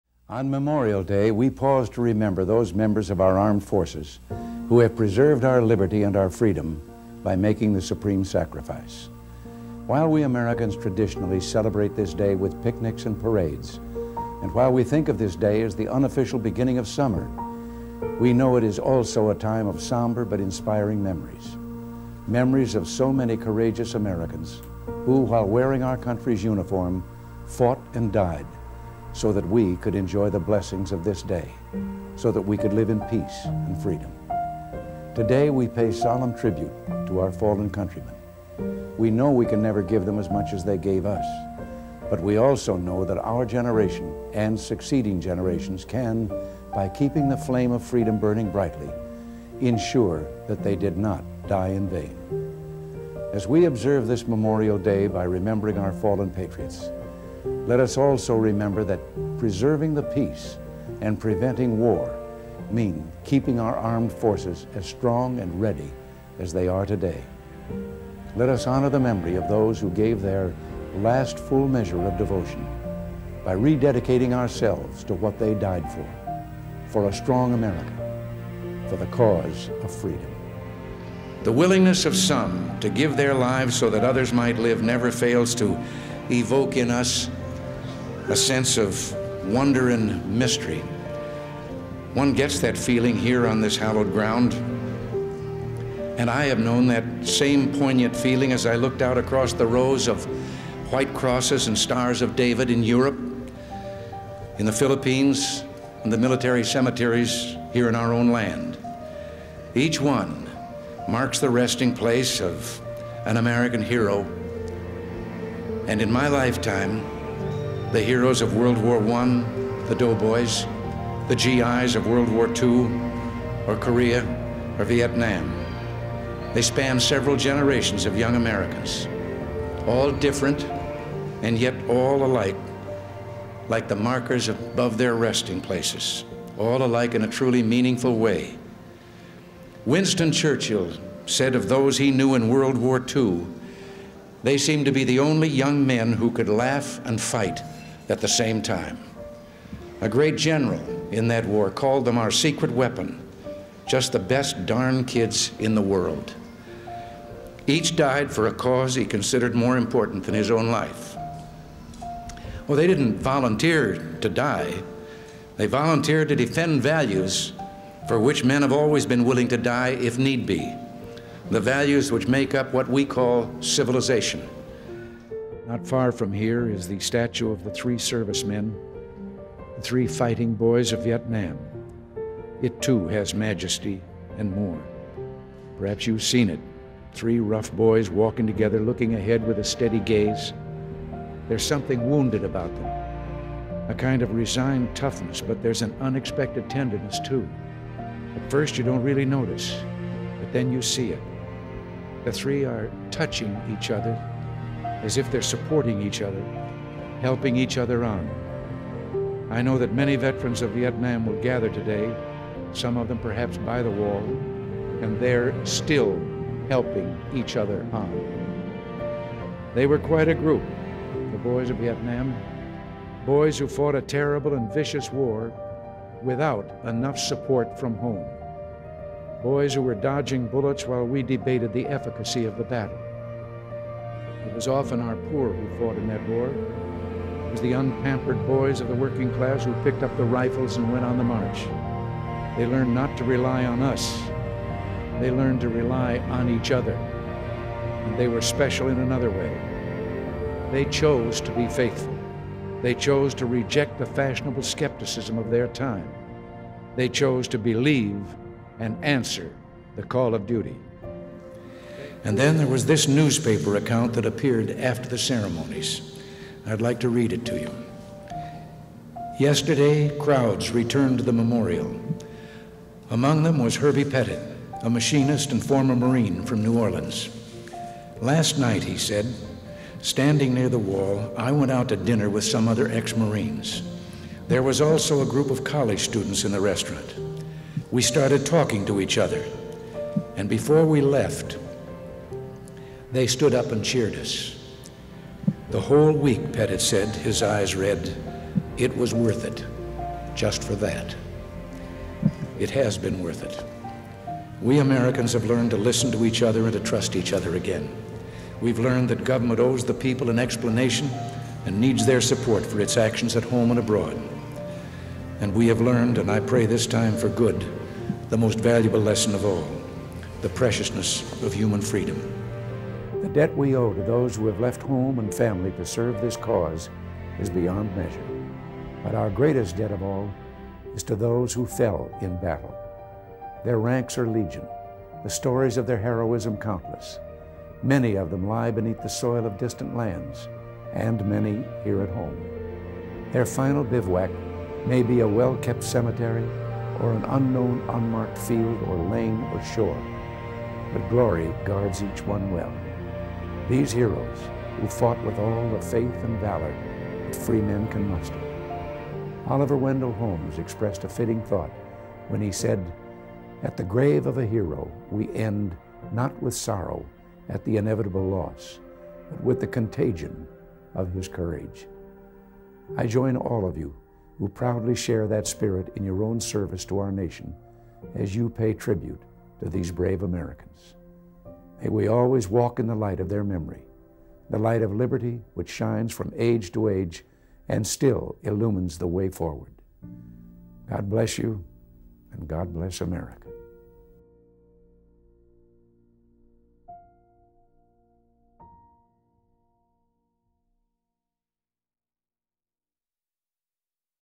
The Best of Ronald Reagan's Memorial Day Speeches: A Tribute to Our Fallen Heroes
President Ronald Reagan was a masterful orator, and his Memorial Day speeches stand out as some of his most moving and inspiring addresses. In this video, we've compiled some of the most memorable moments from President Reagan's Memorial Day speeches over the years. From poignant reflections on the sacrifices of our fallen heroes to stirring calls to action for a more peaceful world, President Reagan's words continue to resonate today. Join us for a powerful tribute to one of America's greatest presidents and his enduring legacy of patriotism and service.